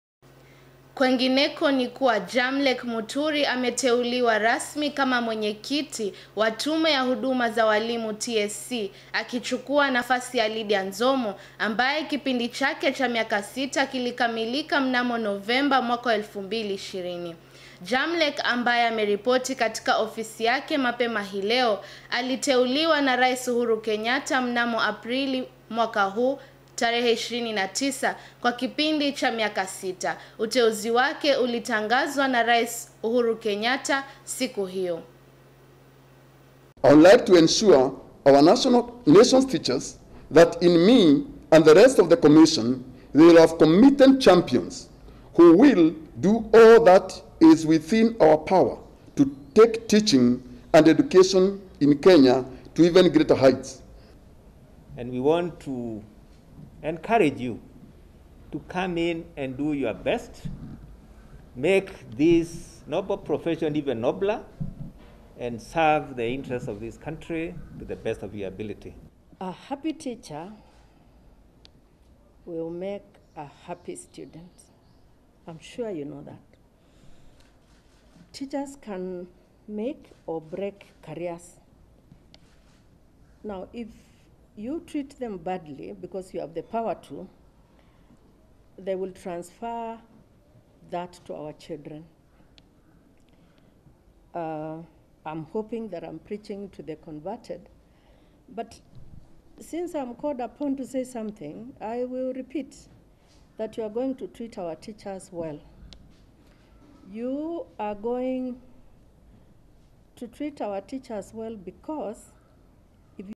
JAMLECK ATEULIWA MWENYEKITI WA TSC
Jamleck Muturi ameteuliwa rasmi kama Mwenyekiti wa Tume ya Huduma za Walimu TSC, akichukua nafasi ya Lydia Nzomo ambaye kipindi chake cha miaka sita kilikamilika mnamo Novemba mwaka wa 2020. Jamleck ambaye ameripoti katika ofisi yake mapema hii leo  aliteuliwa na Rais Uhuru Kenyatta mnamo Aprili 29 kwa kipindi cha miaka sita. Uteuzi wake ulitangazwa na Rais Uhuru Kenyatta mnamo Aprili 29 kwa kipindi cha miaka sita.